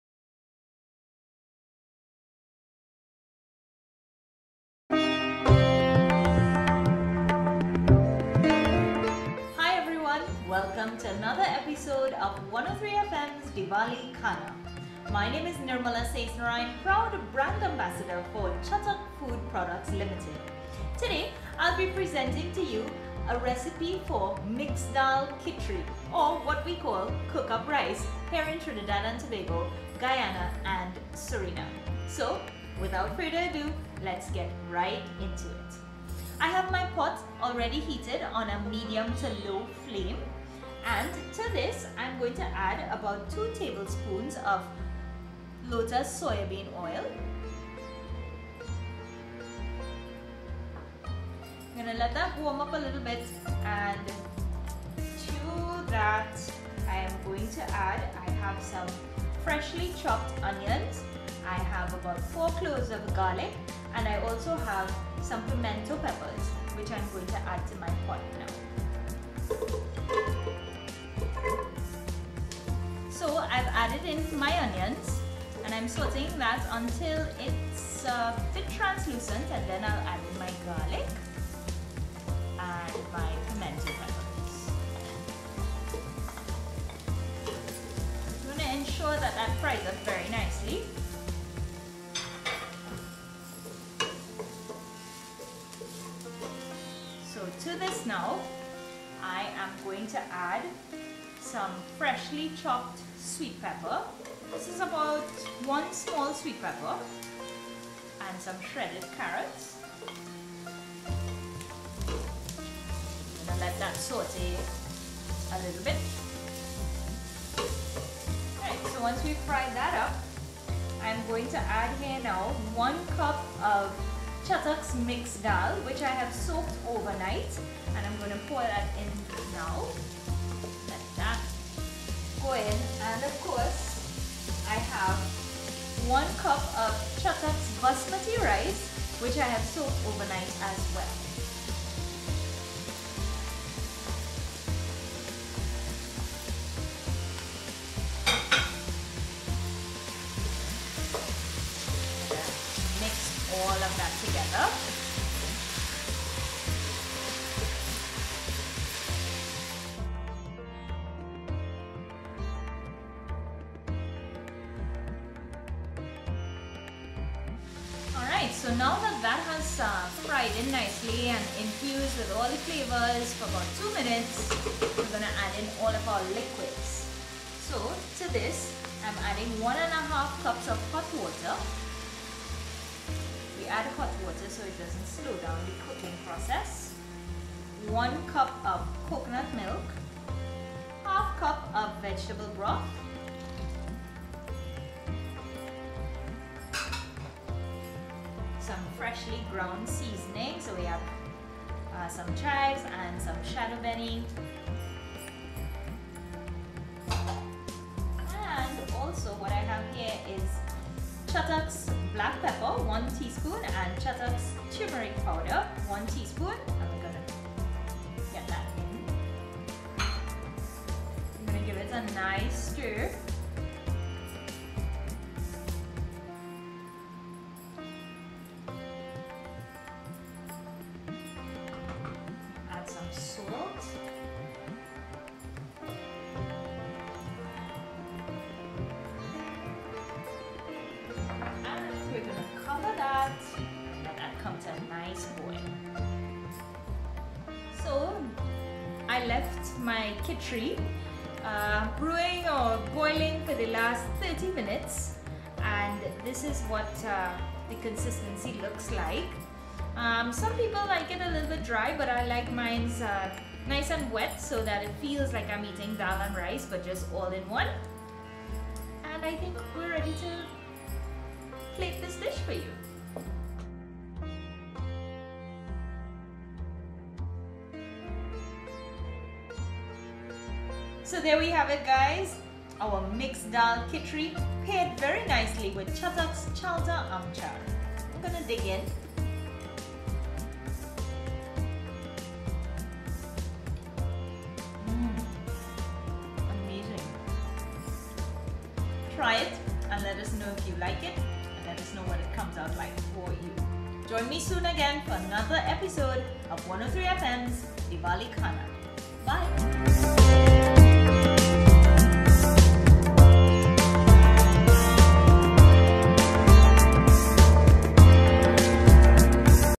Divali Khaana - Savoury Dishes #6 - Dhali Kitchari
Tantalize your taste buds with our online food series "Divali Khaana". In Episode #6 of the savoury dishes, Nirmala Sesnaryan makes a delicious Dhal Kitchari. Watch, enjoy & SHARE!